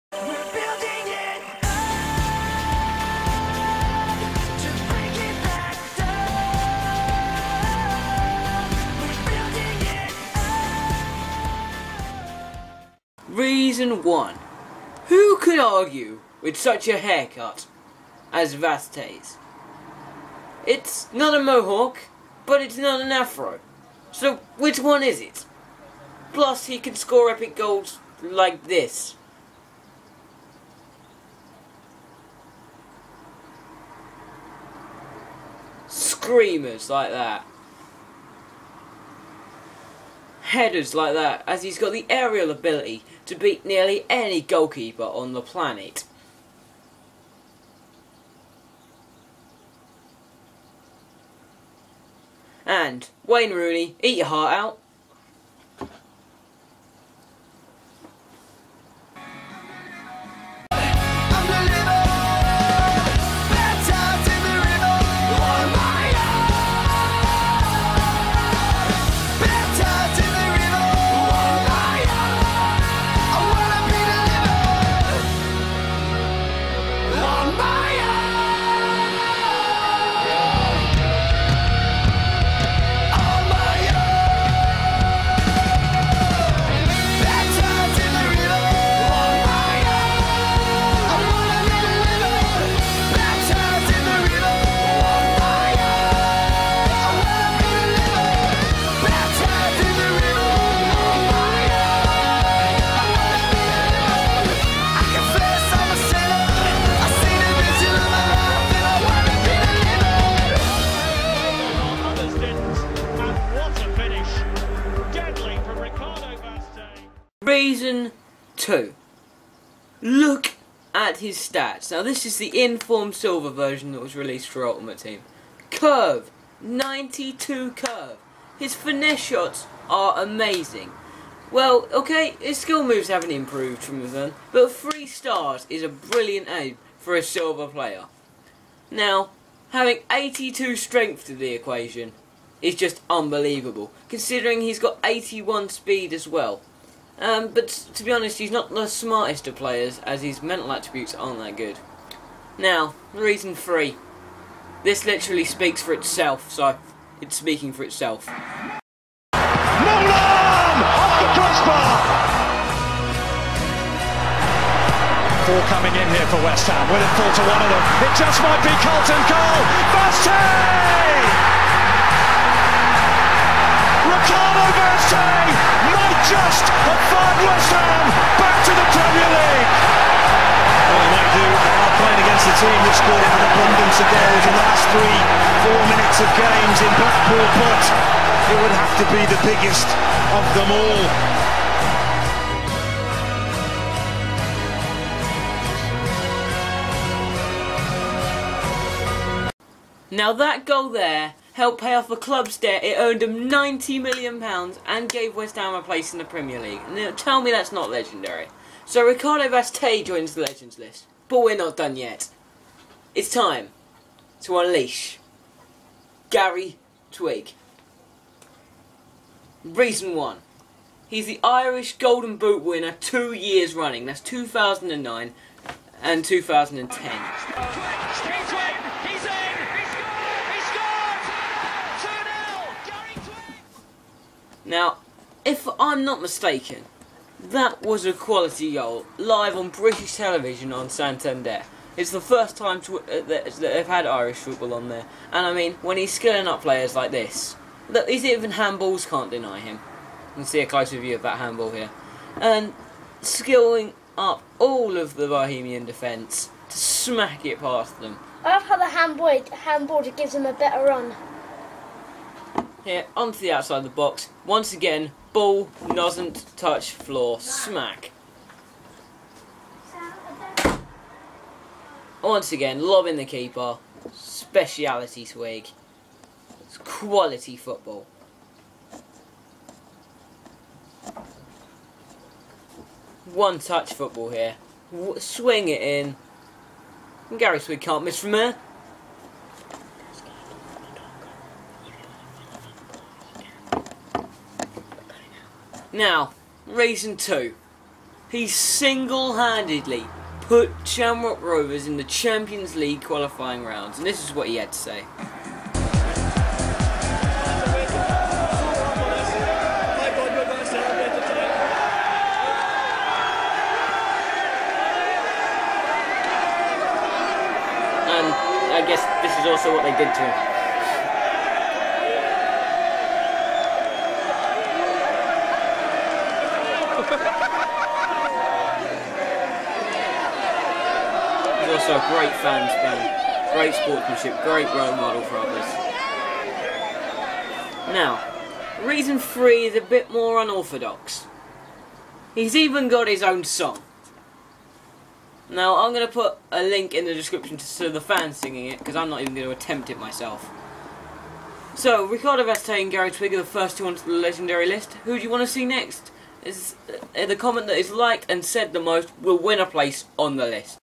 The Legendary List 1
THE FIRST EDITION OF DADDY HEAD AND CHEVEREDLEGEND'S LEGENDARY LIST FEATURING GARY TWIGG AND RICARDO VAZ TE